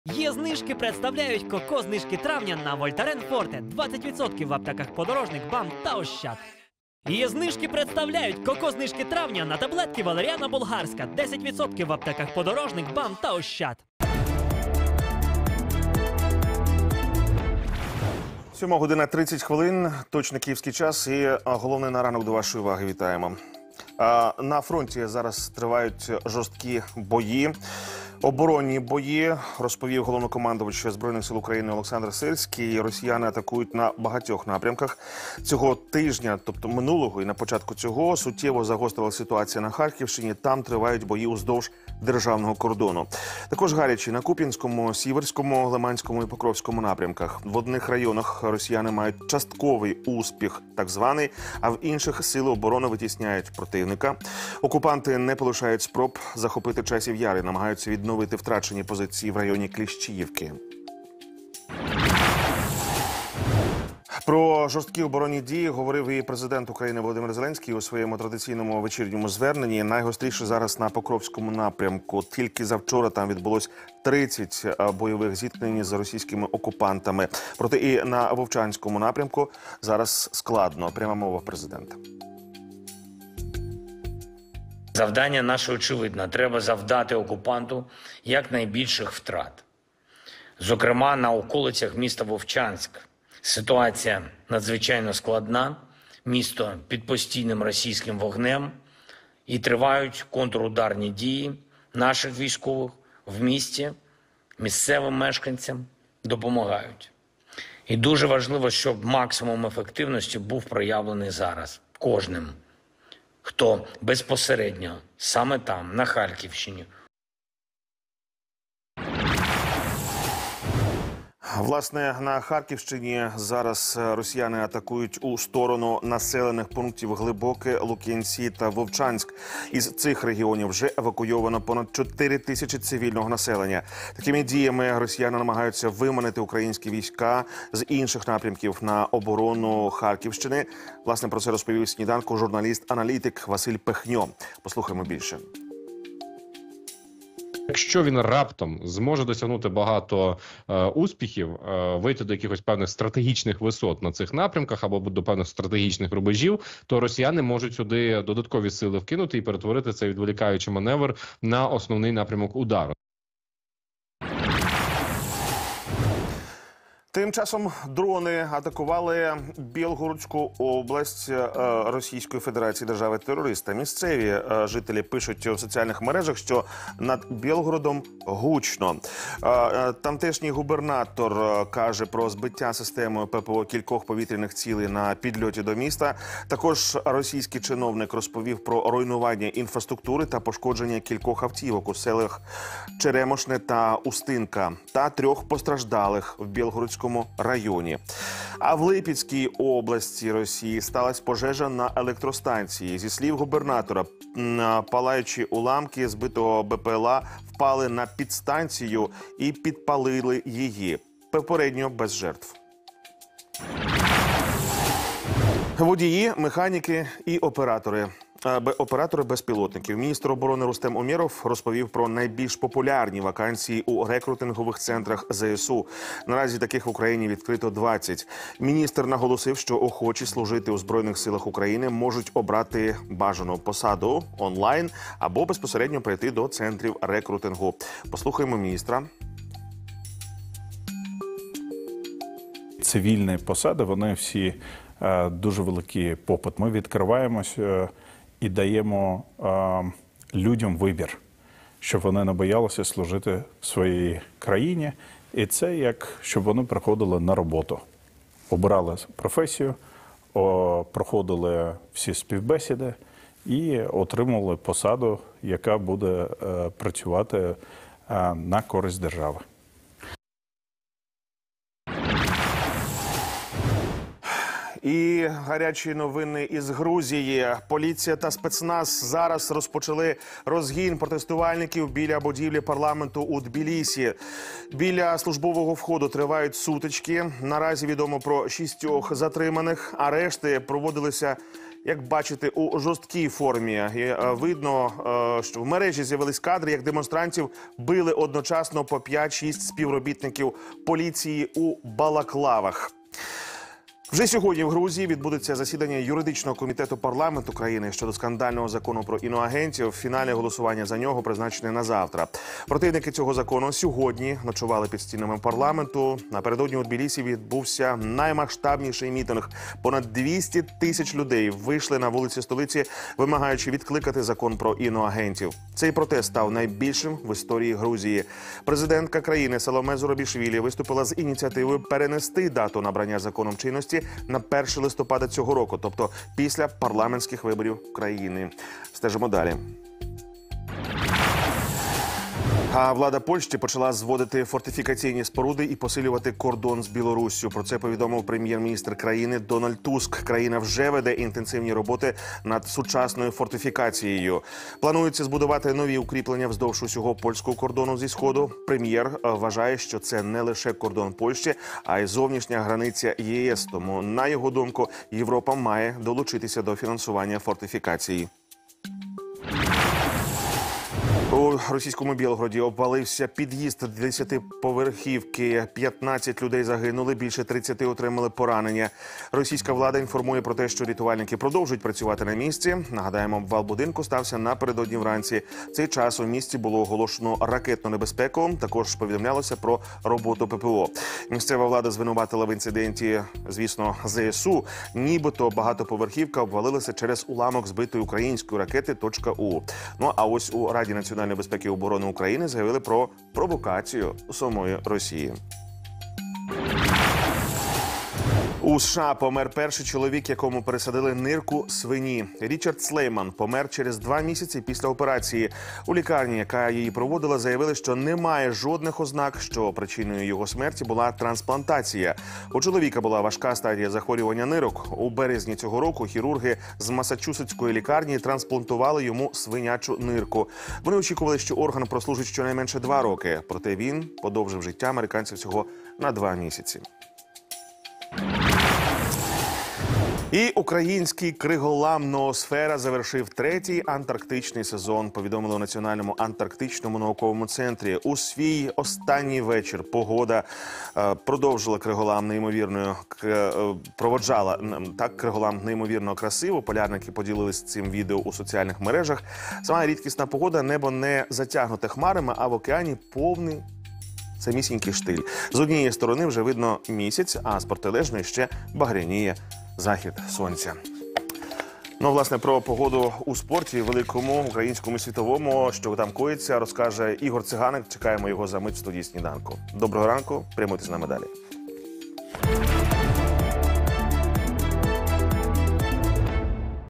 ⚡Головне на ранок 13 травня: Жорстокі бої на фронті, Польща готується до війни, вибухи в Бєлгороді
Головні новини на ранок 13 травня! На фронті тривають жорсткі оборонні бої - розповів головнокомандувач ЗСУ Олександр Сирський. Росіяни атакують на багатьох напрямках. Цього тижня суттєво загострилась ситуації на Харківщині — там тривають бої уздовж державного кордону.На Харківщині ворог атакує у сторону населених пунктів Глибоке, Лукянці та Вовчанськ. Дрони атакували Бєлгородську область. Місцеві жителі пишуть в соцмережах, що над Бєлгородом - "гучно". Влада Польщі почала зводити фортифікаційні споруди й посилювати кордон з Білоруссю. Про це повідомив прем’єр-міністр країни Дональд Туск. Більше подробиць дивіться в сюжеті.